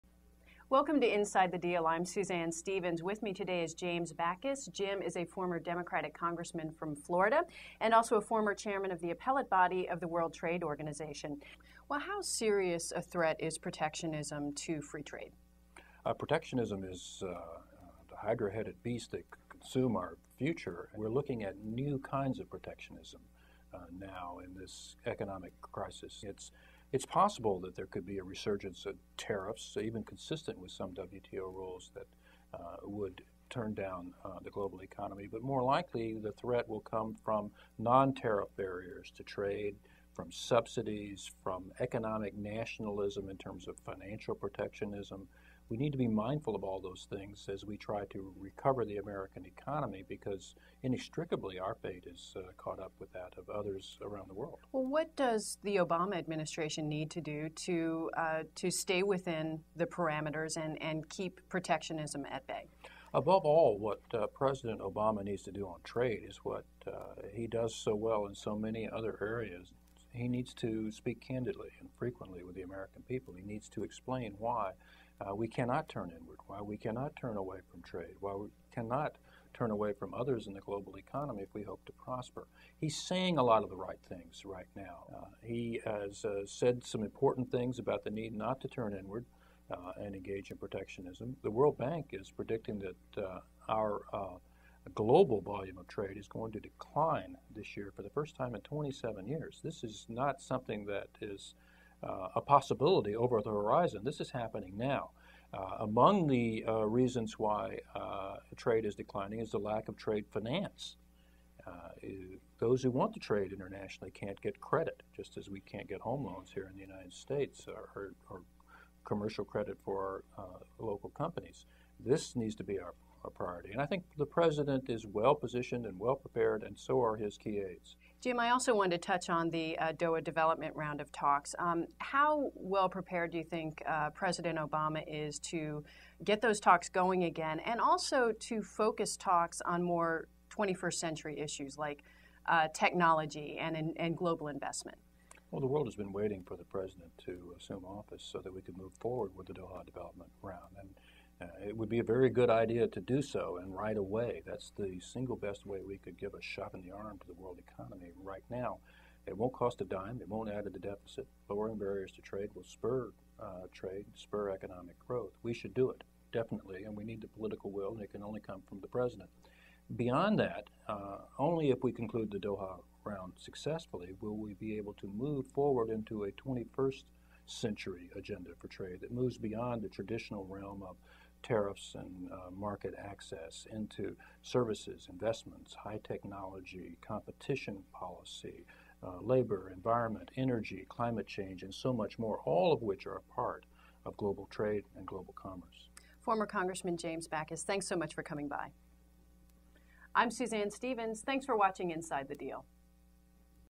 Former Congressman James Bacchus on protectionist dangers
President Barack Obama managed to soften the "buy American" provisions in the economic stimulus bill. Former Democratic Congressman James Bacchus wishes he'd been able to eliminate them altogether. In this edition of Inside the Deal, Bacchus, who chairs the global trade and investment practice group at Greenberg Traurig LLP, tells The Deal's Suzanne Stevens that protectionism poses a serious threat to free trade, and it's up to the President to keep it at bay.